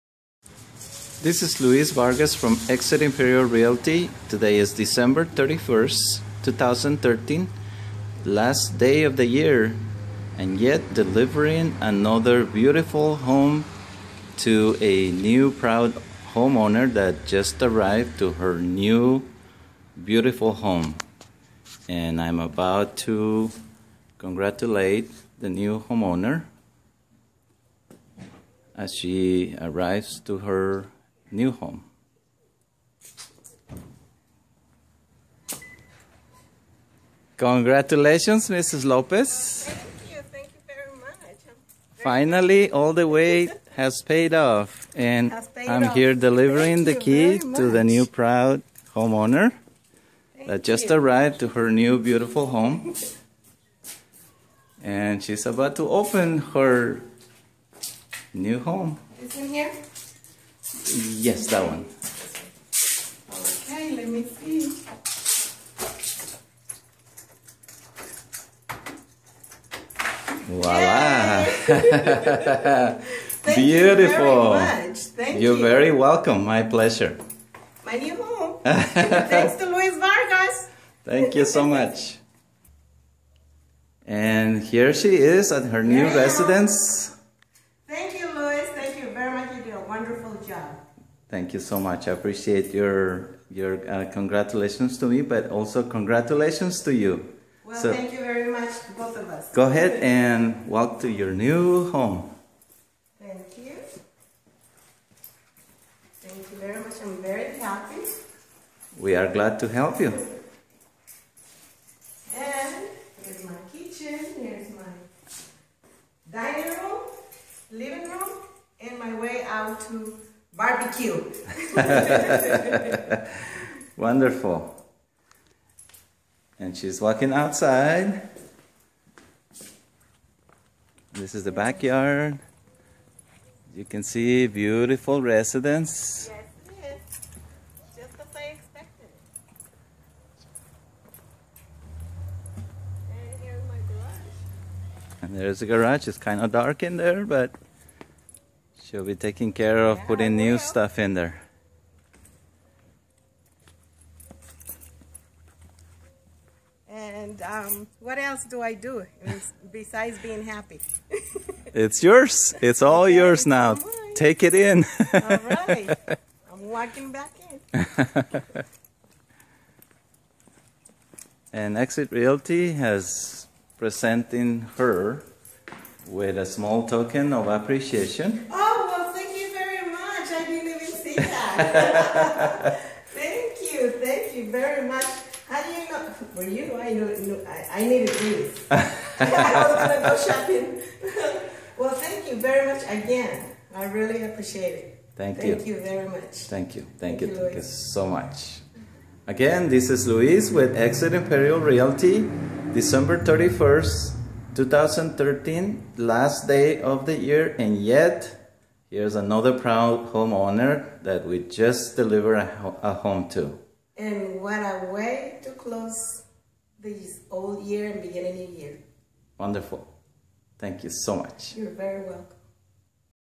Mrs. Lopez new home delivery
Here's another happy client receiving her new home, right on the last day of 2013.